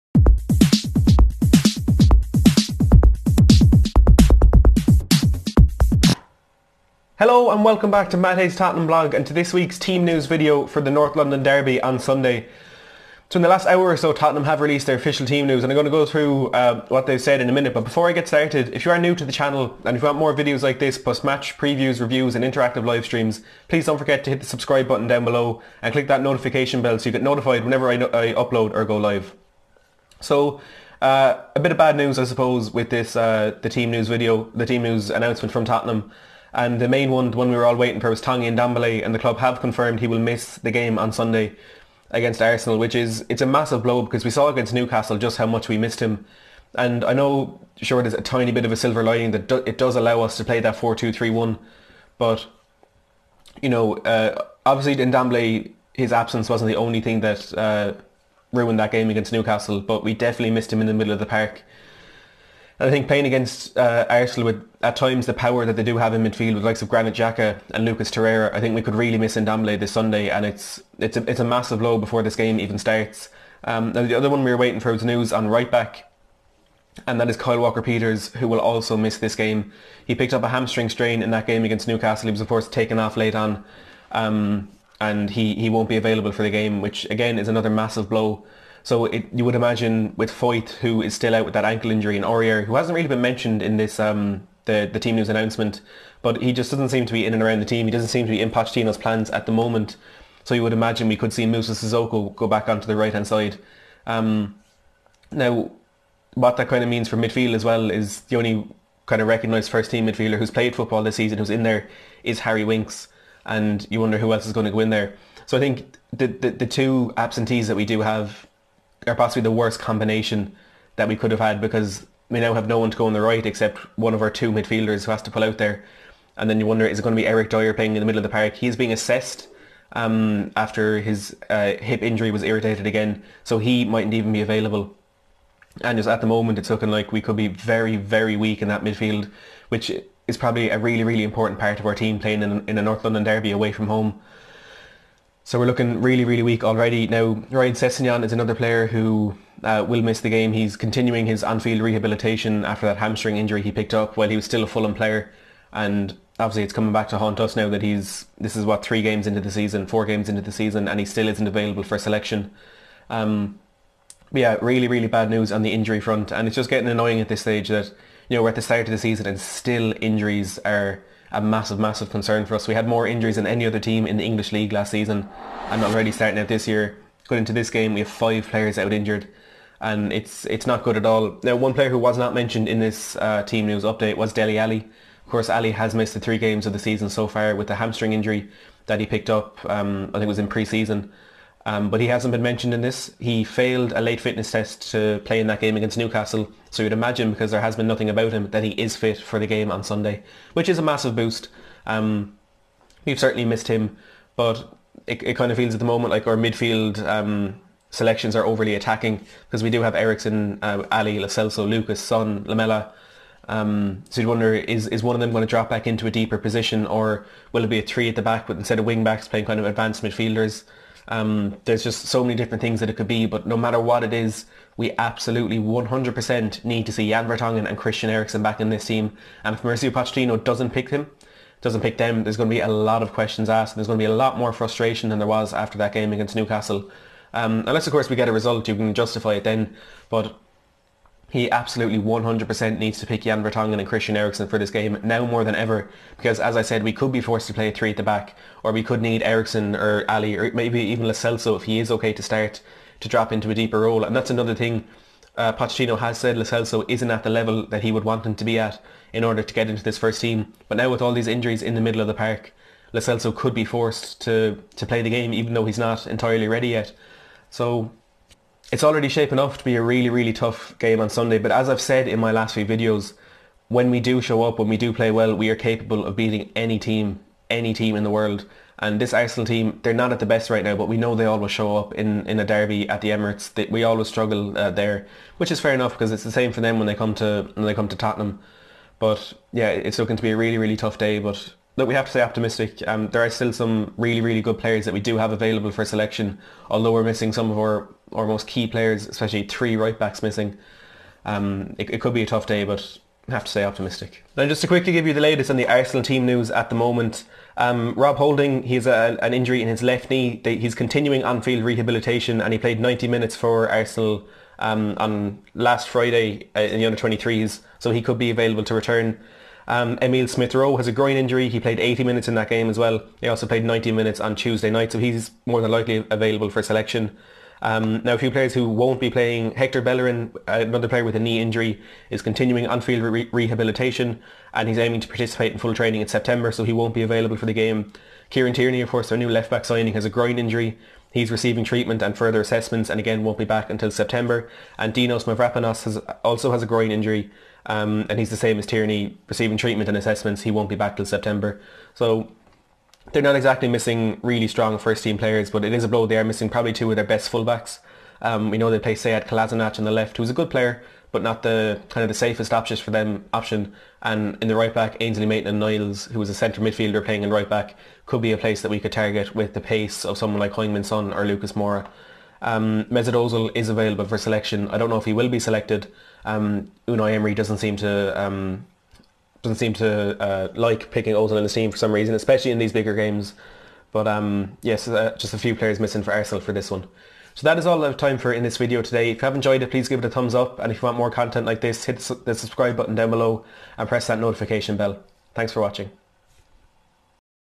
Arsenal vs Tottenham | Team News | Ndombele and KWP Updates
Arsenal vs Tottenham | Team News

Here's the team news from Tottenham's North London Derby clash with Arsenal at the Emirates Stadium on Sunday.